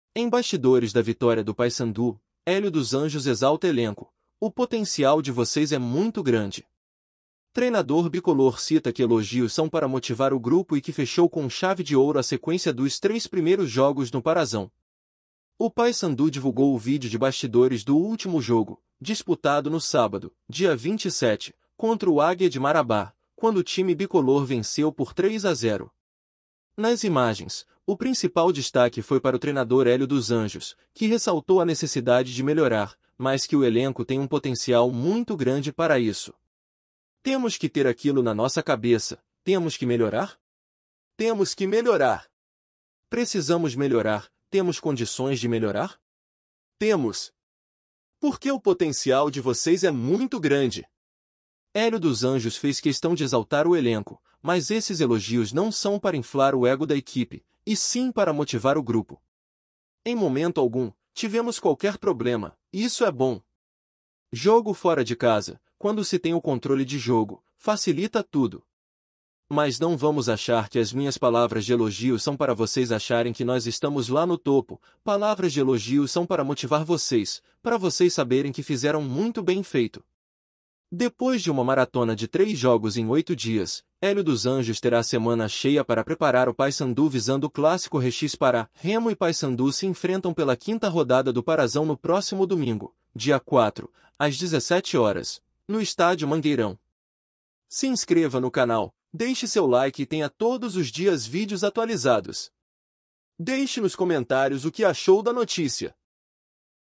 REVELOU TUDO O QUE TA RALANDO NA CURUZU | PAYSANDU NEWS
Em bastidores da vitória do Paysandu, Hélio dos Anjos exalta elenco: “O potencial de vocês é muito grande”

Treinador bicolor cita que elogios são para motivar o grupo e que fechou com chave de ouro a sequência dos três primeiros jogos no Parazão

O Paysandu divulgou o vídeo de bastidores do último jogo, disputado no sábado, dia 27, contra o Águia de Marabá, quando o time bicolor venceu por 3 a 0. Nas imagens, o principal destaque foi para o treinador Hélio dos Anjos, que ressaltou a necessidade de melhorar, mas que o elenco tem um potencial “muito grande” para isso.

- Temos que ter aquilo na nossa cabeça, temos que melhorar? Temos que melhorar! Precisamos melhorar, temos condições de melhorar? Temos! Porque o potencial de vocês é muito grande!

Hélio dos Anjos fez questão de exaltar o elenco, mas esses elogios não são para inflar o ego da equipe, e sim para motivar o grupo.

- Em momento algum, tivemos qualquer problema, isso é bom! Jogo fora de casa, quando se tem o controle de jogo, facilita tudo. Mas não vamos achar que as minhas palavras de elogios são para vocês acharem que nós estamos lá no topo, palavras de elogios são para motivar vocês, para vocês saberem que fizeram muito bem feito.

Depois de uma maratona de três jogos em oito dias, Hélio dos Anjos terá a semana cheia para preparar o Paysandu visando o clássico RE x PA. Remo e Paysandu se enfrentam pela quinta rodada do Parazão no próximo domingo, dia 4, às 17h, no Estádio Mangueirão.

DEIXE NOS COMENTÁRIOS O QUE ACHOU DA NOTÍCIA!